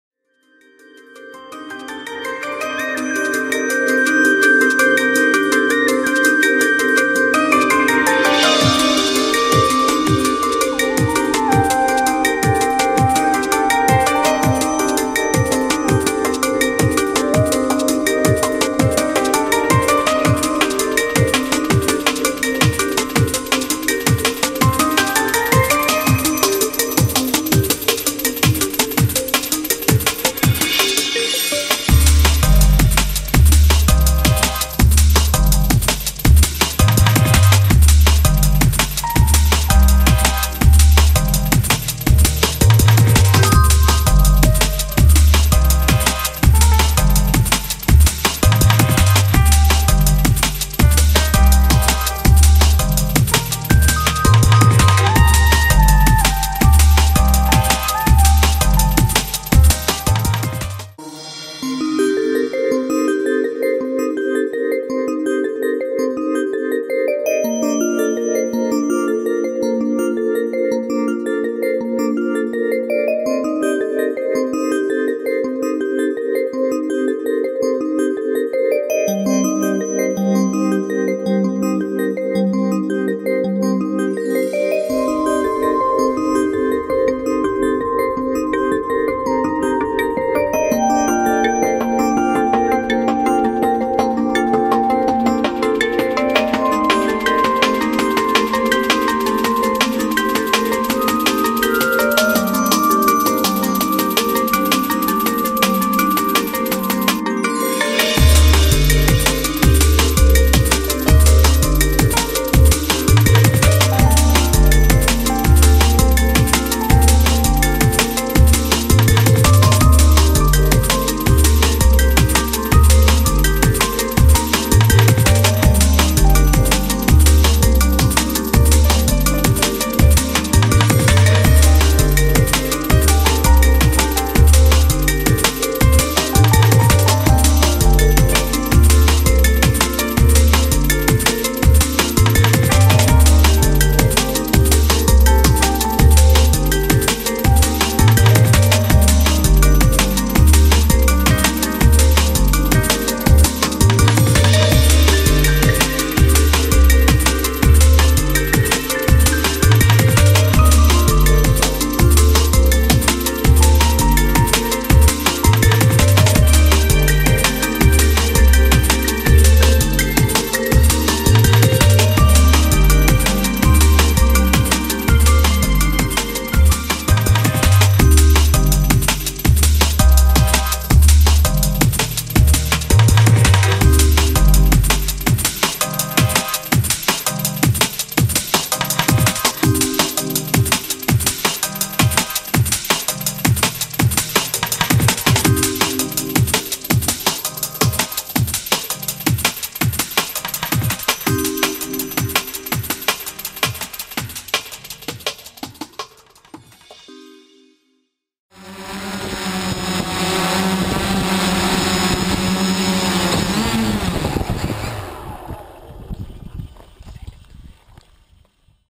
Autumn vol.2
Tonight was the first severe frost. -5C degrees all night. Now one of the windiest day, and the trees are bare. And then snow, snow, snow ... I tried use one last opportunity, to catch the last colors.
By the way, many meteorological centers predicting very cold winter in Northern Europe in this year :)
fpv.levi.ee